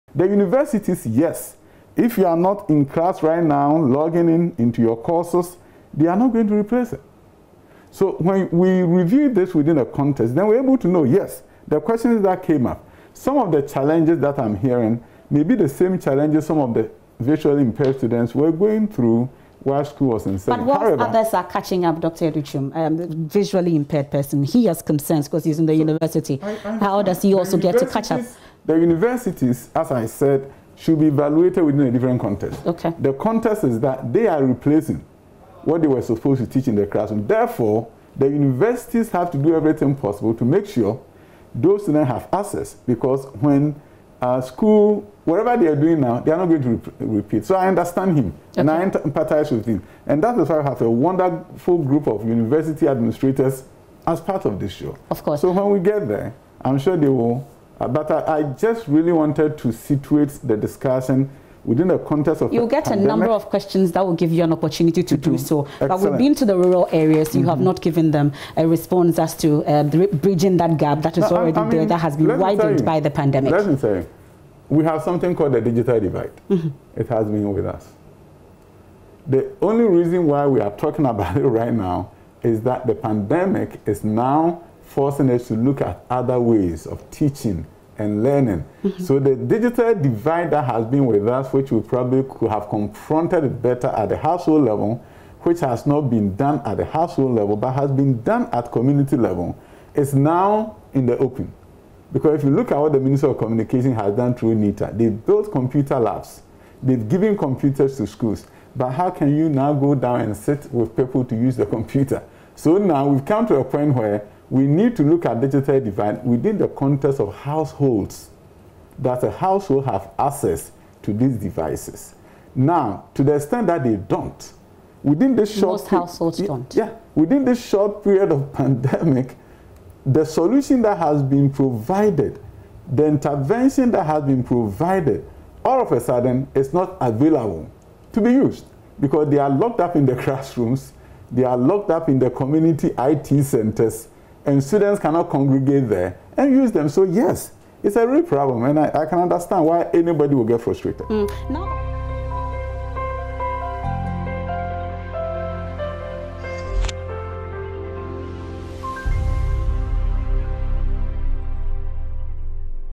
COVID19 AND EDUCATION: Pandemic now forcing us to look at other ways of teaching - Yaw Adu Twum
COVID19 AND EDUCATION: The pandemic is now forcing us to look at other ways of teaching and learning - Deputy Education Minister, Yaw Adu Twum